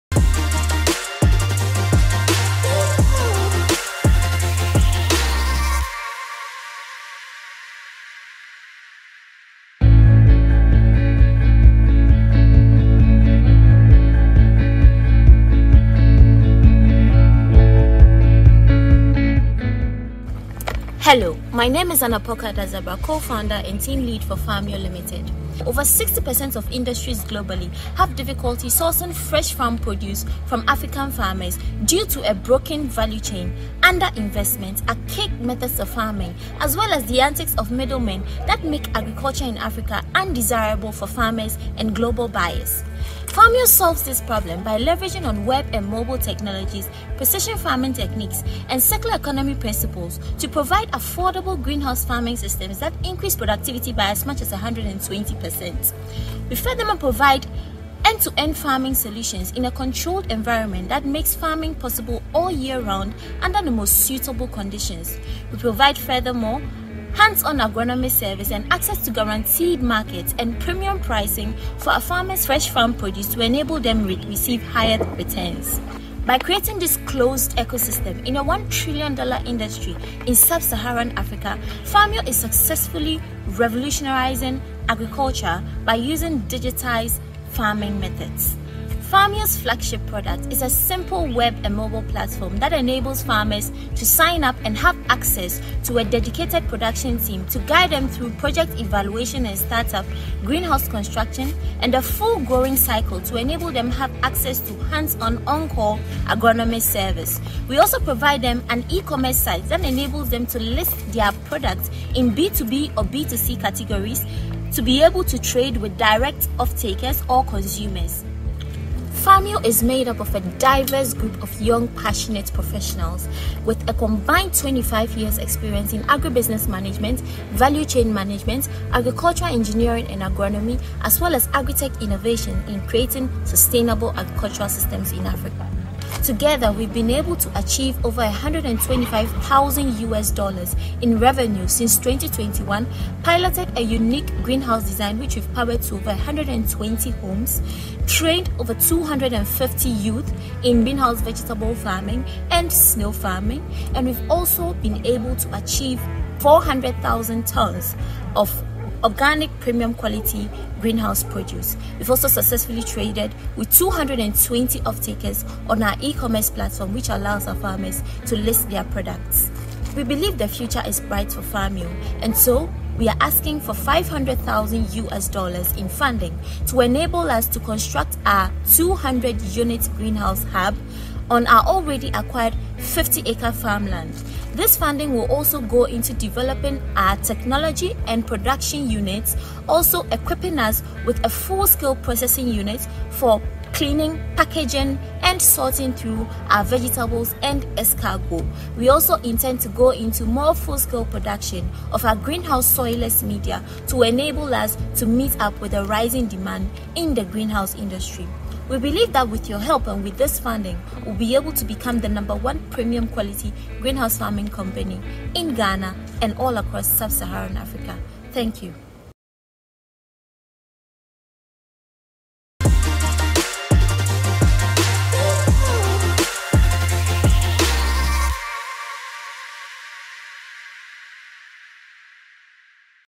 WE4A Pitch | Farmio Limited
Farmio leverages precision farming techniques, circular economy principles and agri-tech solutions to provide affordable greenhouse farms to guarantee about 120% increased productivity.

We provide end-to-end farming solutions in affordable, ecofriendly greenhouse farming systems which makes farming possible all year round under the most suitable conditions while also providing access to markets for our farmers.

With a sole focus on vegetable and snail greenhouse farming, we have built our business to be highly sustainable and adept at providing farm to fork solutions which the farmer needs to be profitable. We offer full value chain solutions which span from commercial greenhouse farm construction, farm setup, greenhouse farming training and management, on-call agronomist support for all growing seasons and access to premium greenhouse produce buyers.